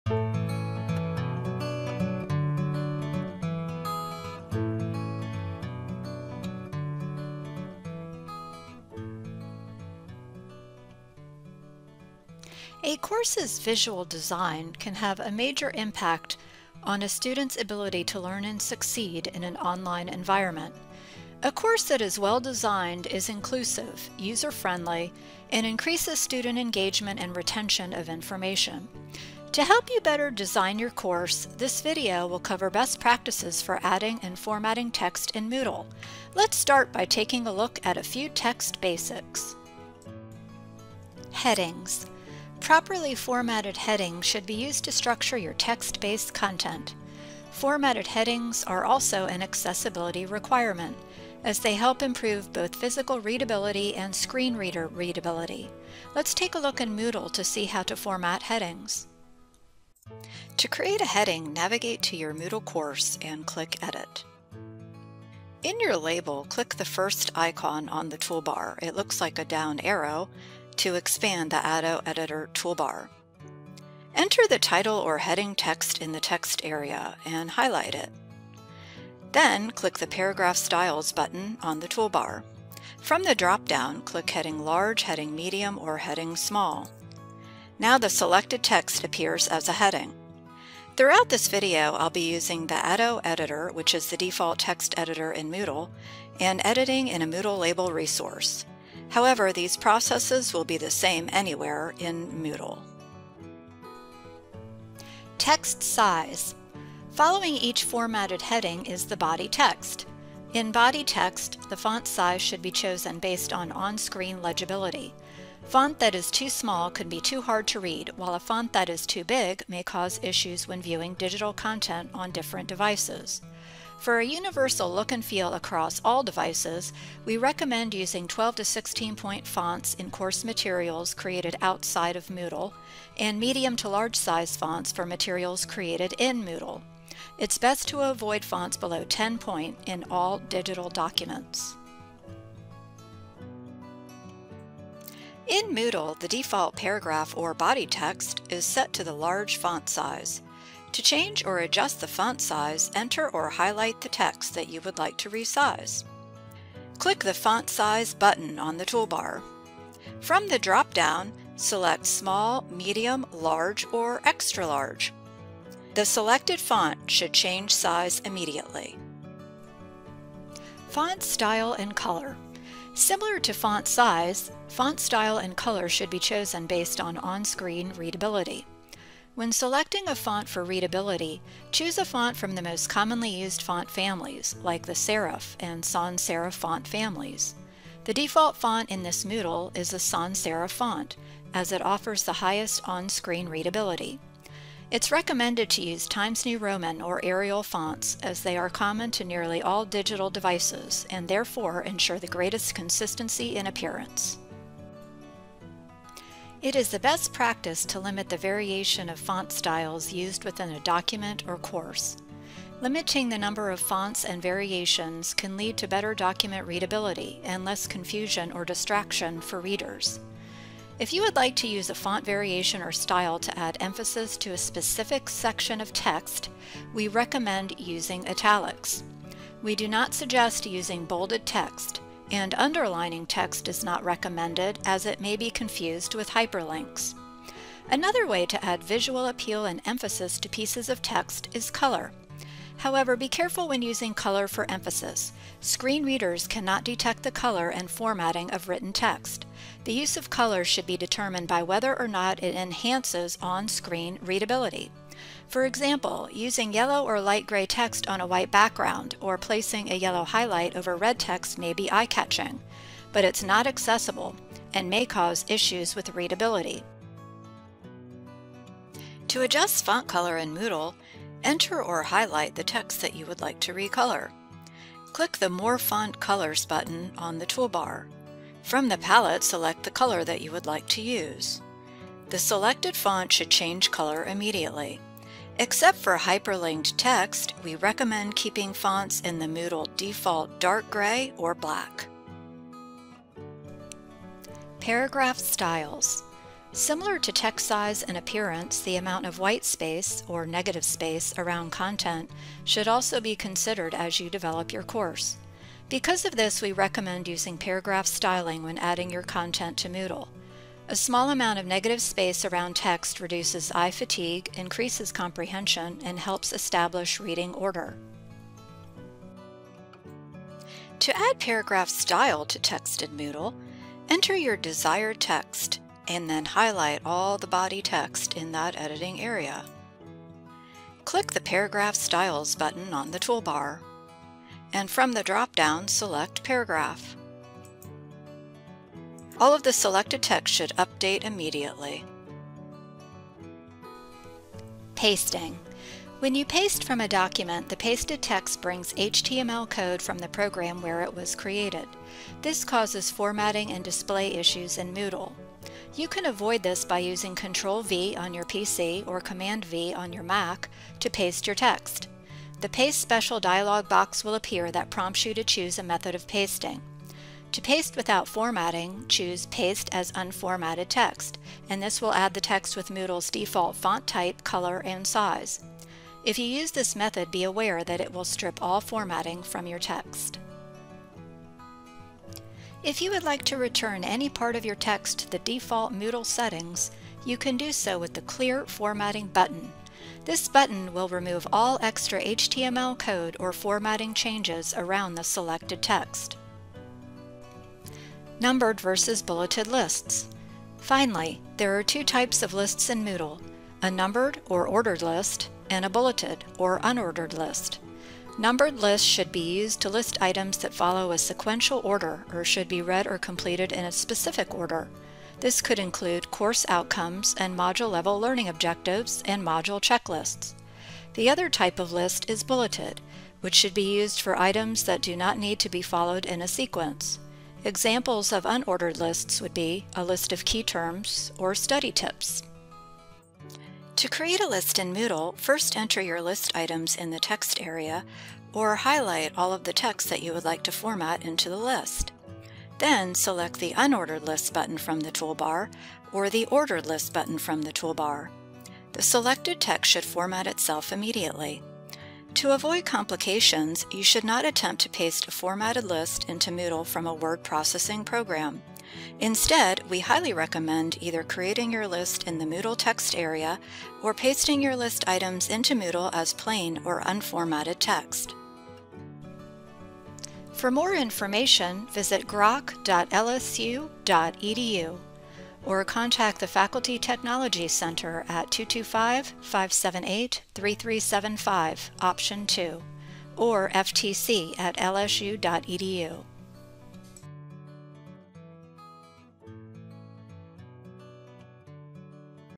Best Practices for Formatting Text in Moodle
The appearance of your course can have a major impact on a student’s ability to learn and succeed in an online environment. A course that is well-designed is inclusive and user-friendly and can contribute to increased student engagement and retention of information, as well as provide a learning environment that is accessible for all learners. In this video, we will cover best practices for adding and formatting text in Moodle.